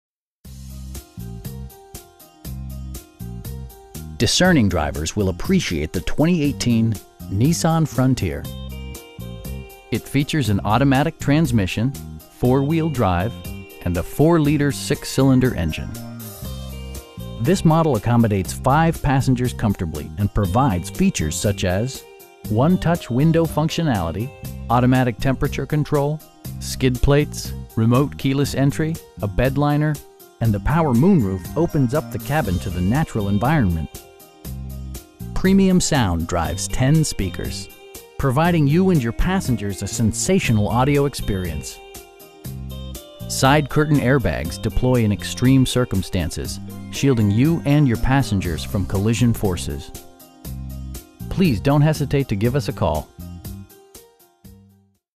2018 Nissan Frontier PRO-4X in Fredericton, NB E3B5C8
Fairview Jeep Ram
1065 Hanwell Road

Discerning drivers will appreciate the 2018 Nissan Frontier. It features an automatic transmission, 4-wheel drive, and a 4 liter 6 cylinder engine. This model accommodates 5 passengers comfortably, and provides features such as: 1-touch window functionality, automatic temperature control, skid plates, remote keyless entry, an overhead console, a bedliner, and the power moon roof opens up the cabin to the natural environment. Premium sound drives 10 speakers, providing you and your passengers a sensational audio experience. Side curtain airbags deploy in extreme circumstances, shielding you and your passengers from collision forces. We have the vehicle you've been searching for at a price you can afford. Please don't hesitate to give us a call.